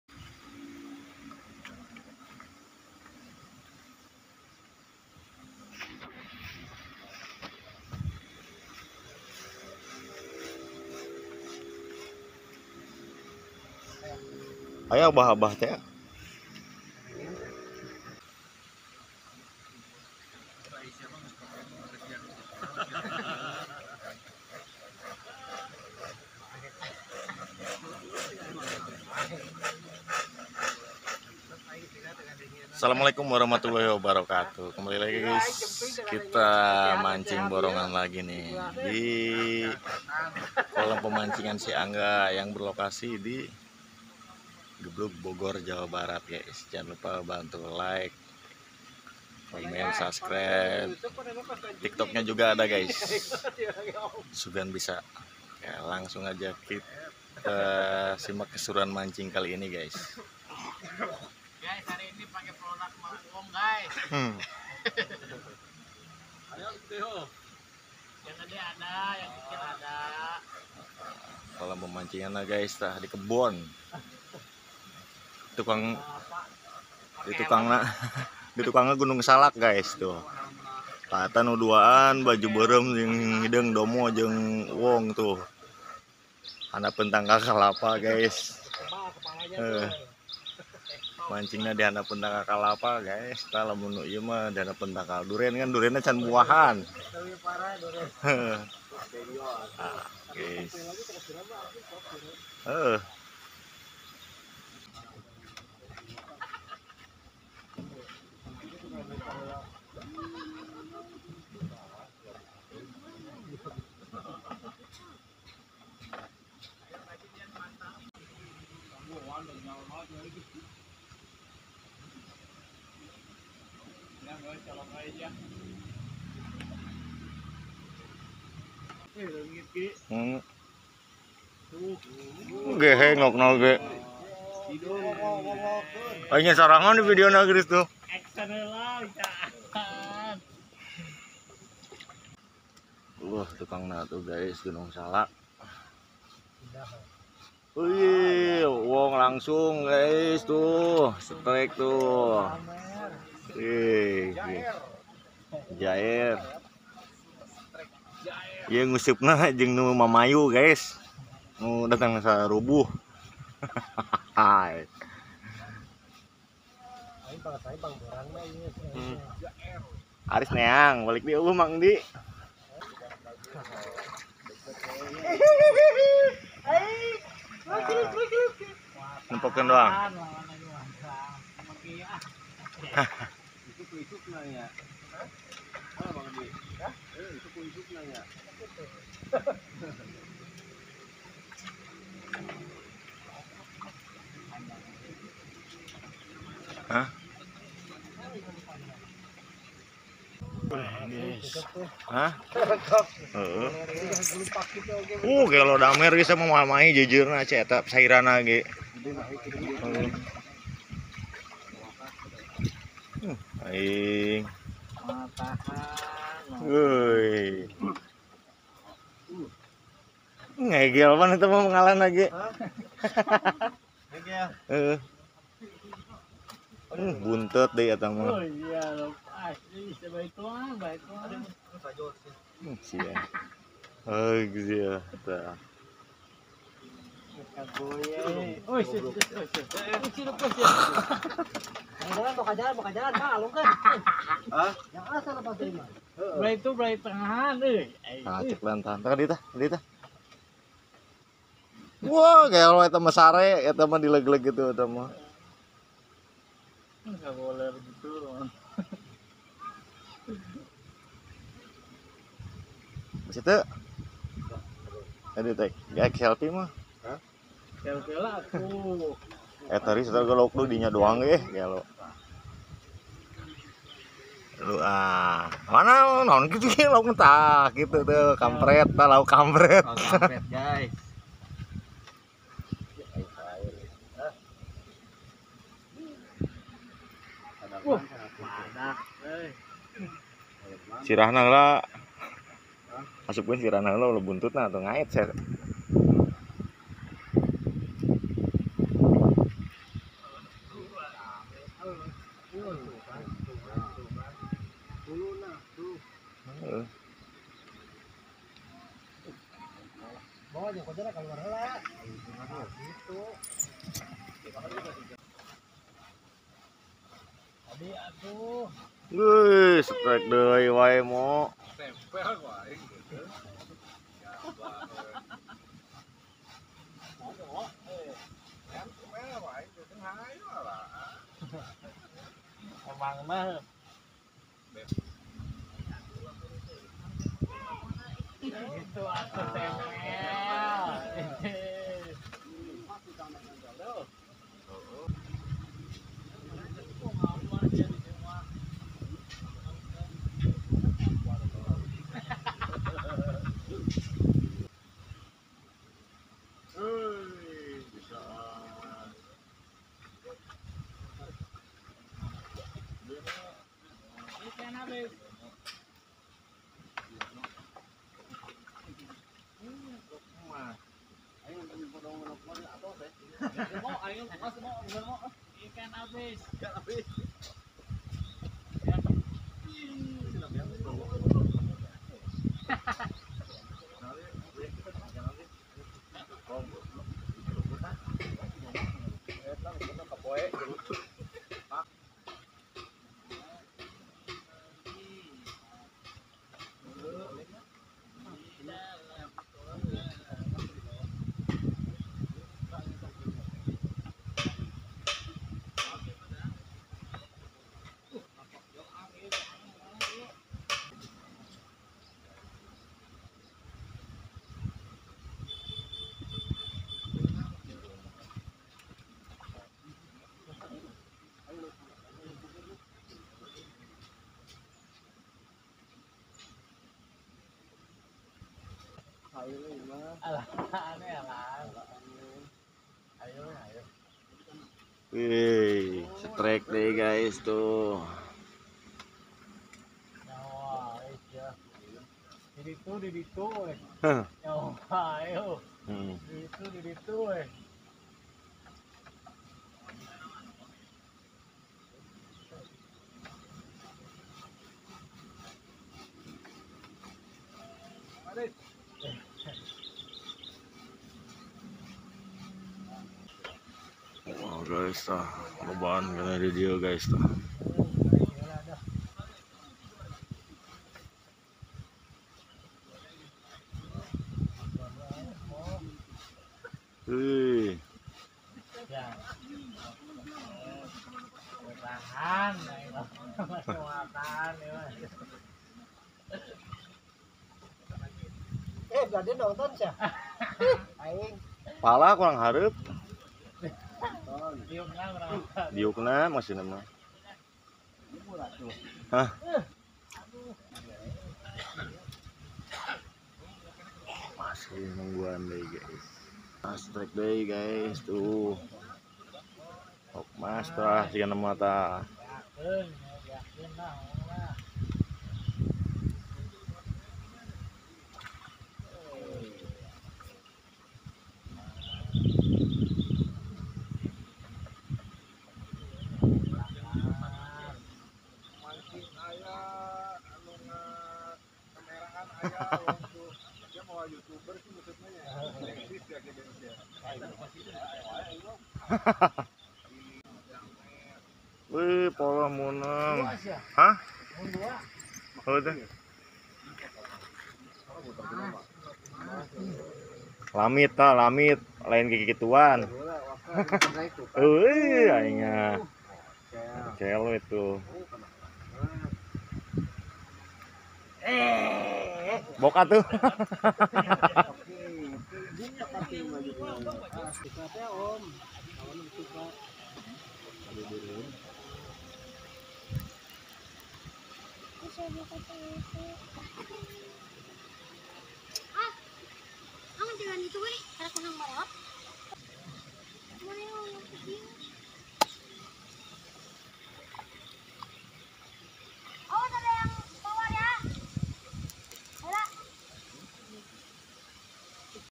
BORONGAN MANCING IKAN NILA DAN IKAN MAS‼️TERGANAS
Assalamualaikum wr'wb.
seperti biasa kita mancing borongan lagi gaes sungguh diluar dugaan ikannya pada ganas ganas. dengan pemandang view gunung salak.